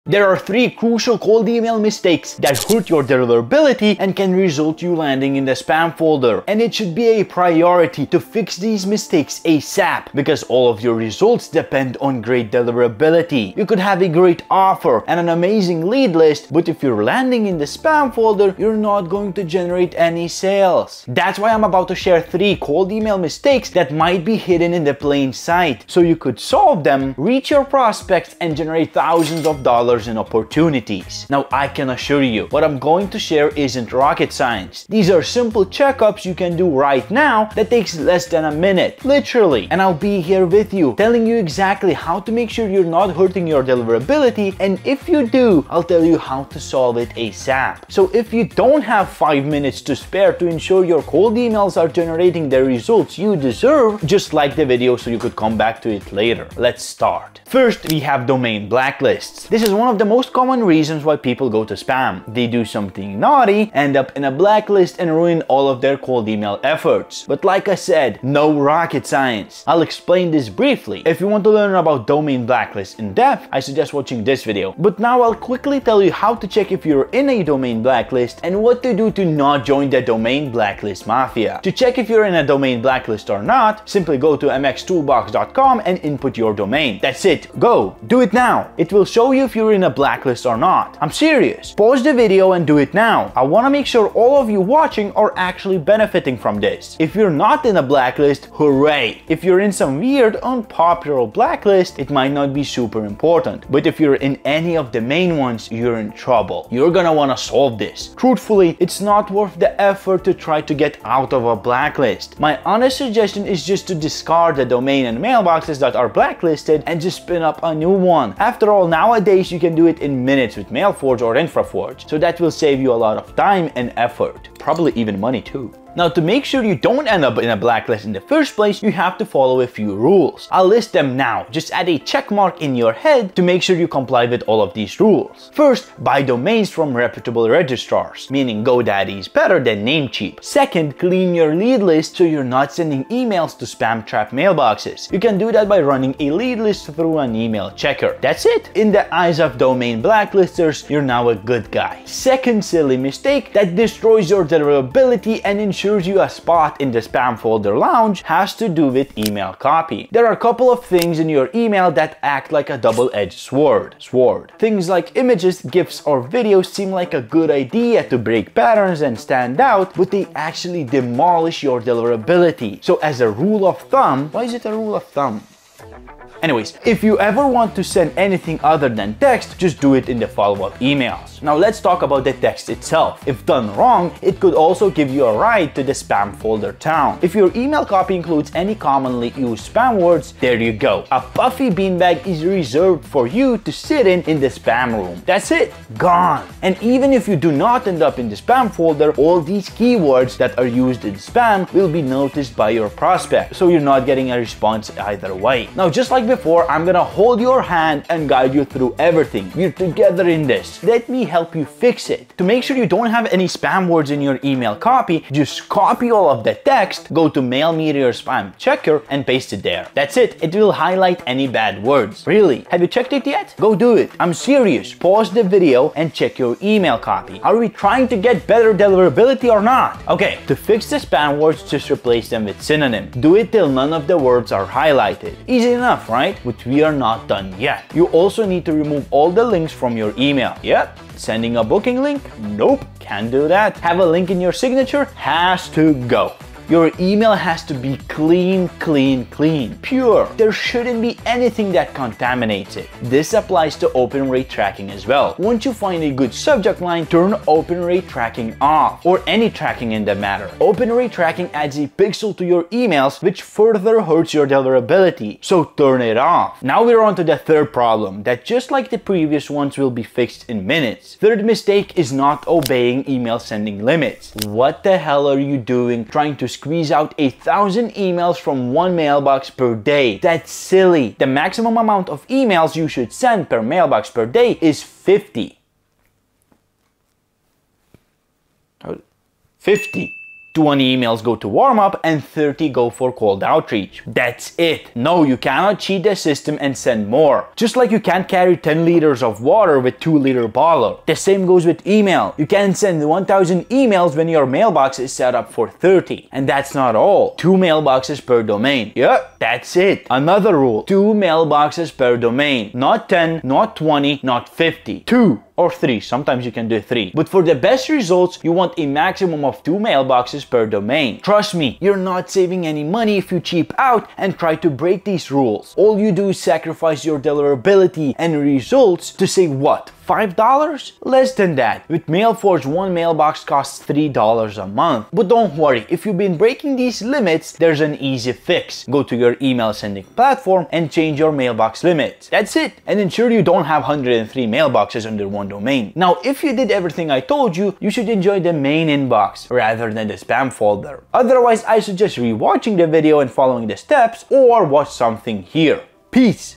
3 Cold Email Mistakes That DESTROY Email Deliverability
Are you struggling with your cold email outreach? In this video, we highlight the **3 biggest cold email mistakes** that could be hurting your email deliverability and overall success. Whether you're a seasoned marketer or just starting, avoiding these pitfalls is crucial for getting your emails into the inbox rather than the spam folder.

Join us as we break down essential email deliverability tips to enhance your cold email strategy. Learn how to craft effective subject lines, personalize your messages, and maintain a healthy sender reputation.

- The top 3 mistakes to avoid in cold emailing
- Tips for improving email deliverability
- Best practices for engaging your audience

Don't let your cold emails go unnoticed! Watch now to ensure your outreach efforts are effective and impactful.

00:00 - Intro
00:57 - Domain Blacklists
02:31 - Email Copy
04:26 - Email Sending Limits